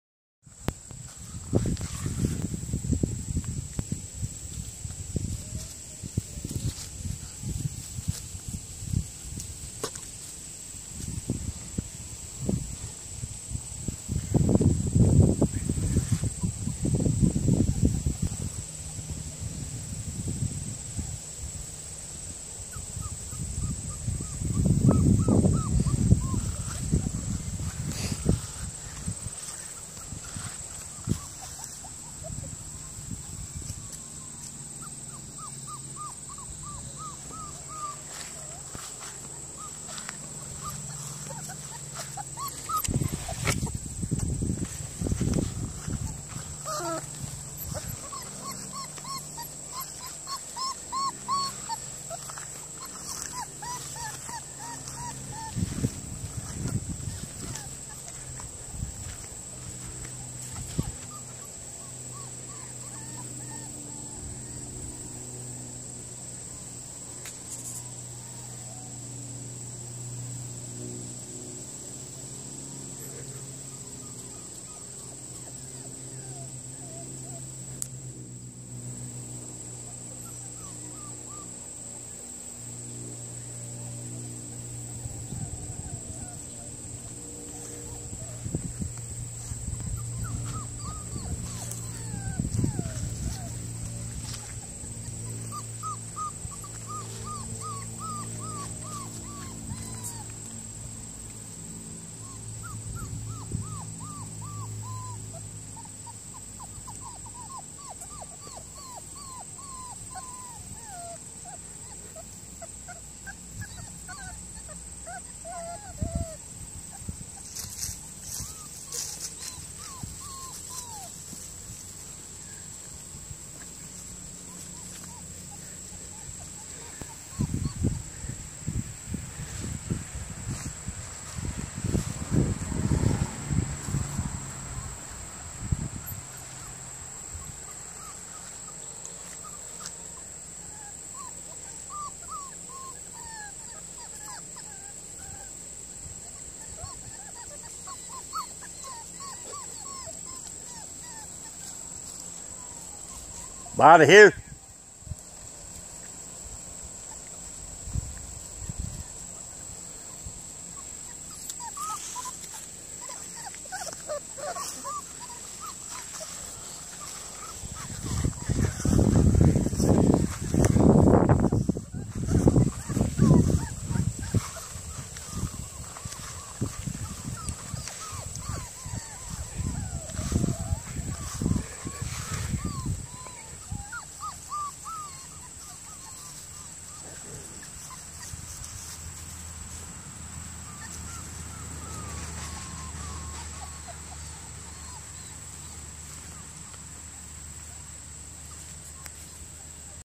Outside this morning
Going outside this morning puppies are still getting used to the big world I had to round 2 up that went exploring